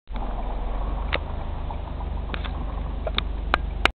Sign of Taylor Swift
Sign of Days Inn in Rapid City saying Taylor Swift stayed there